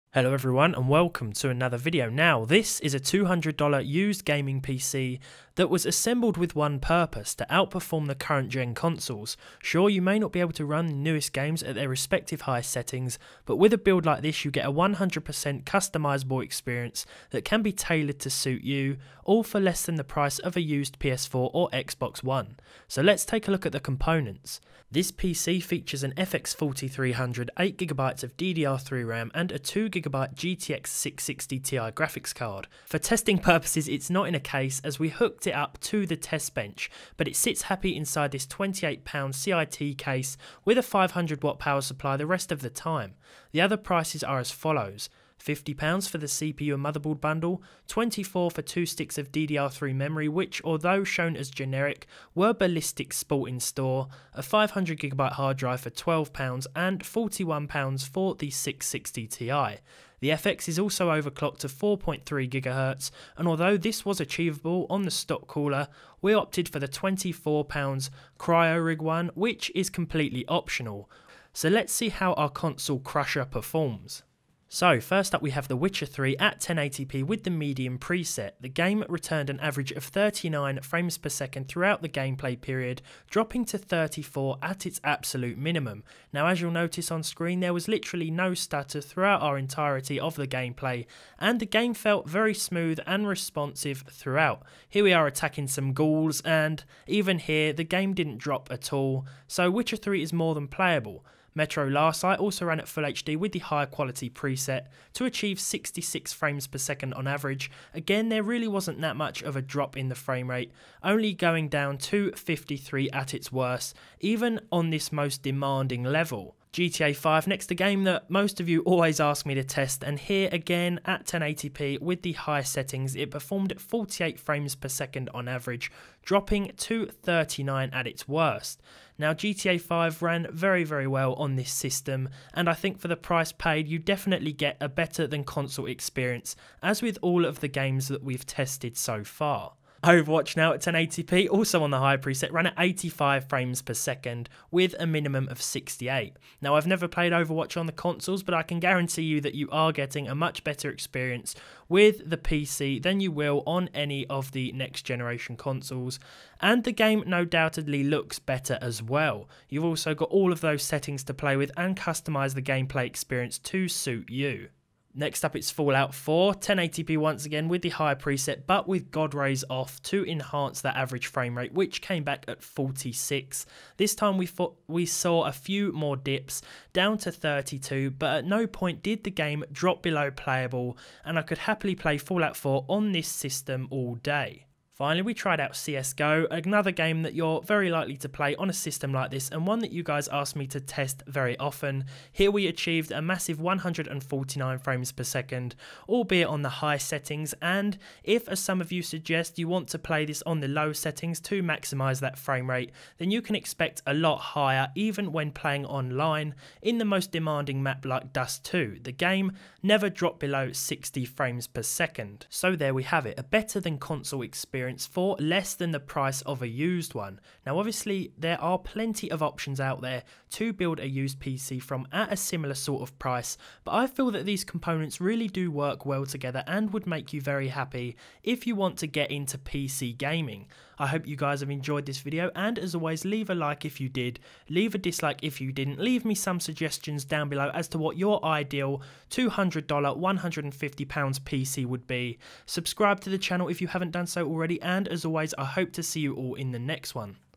An Overclocked "Console Crushing" $200 Budget Gaming PC Build
It's been a while since we've featured a budget gaming PC build on this channel, and with the used market ever changing I thought it would be cool to see what sort of PC we could build for less than the price of a used current gen console, that offers more performance of course. This is the second hand, overclocked $200 Budget System.

Specs:
FX 4300 @ 4.3GHz
GTX 660Ti
8GB DDR3
System tested with Windows 10, which will cost extra of course, but O/S choice is entirely yours :)

Fancy a shirt, hoodie or mug?